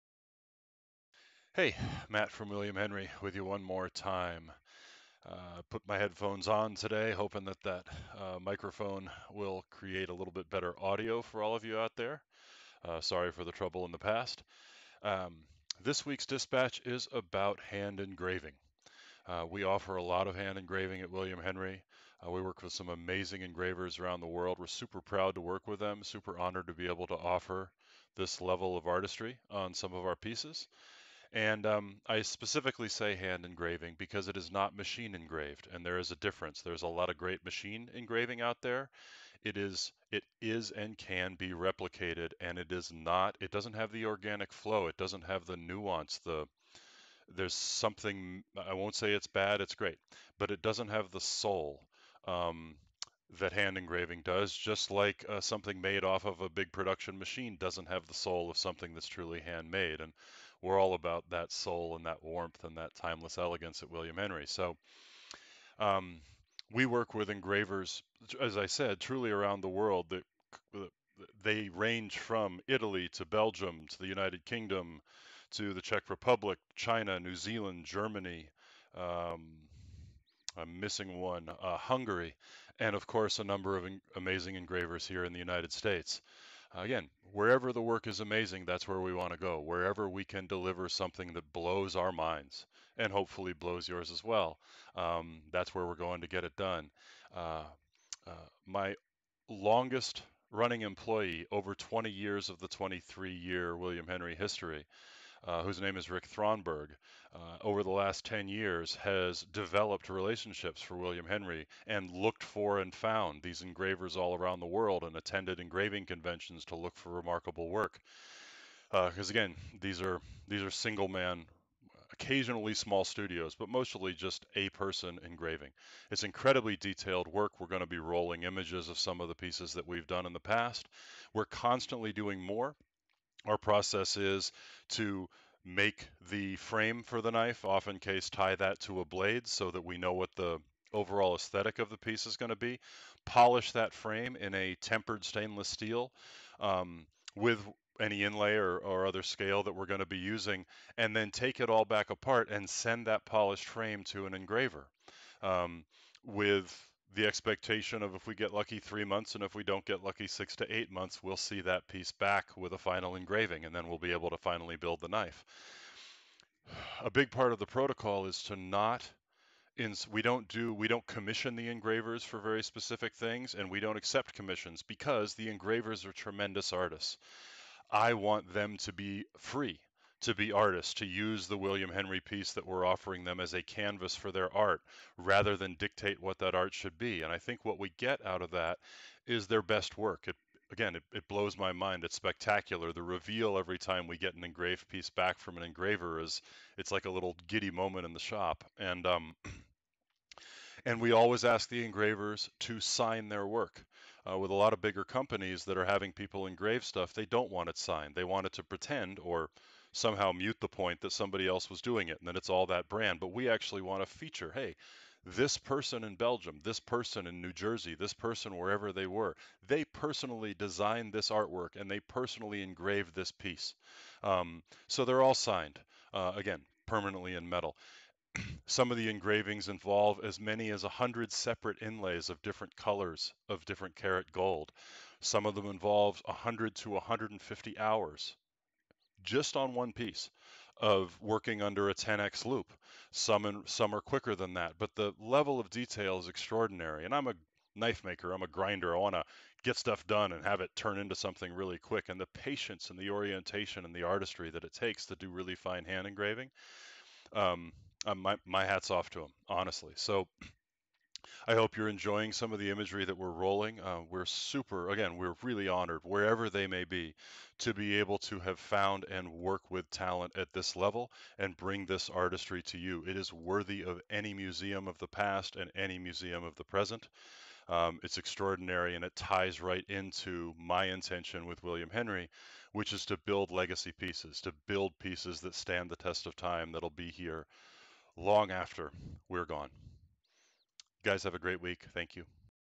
WH Hand-Engraving
Matt Conable speaks about William Henry's most exclusive knives and the amazing artistry of our hand-engraved work.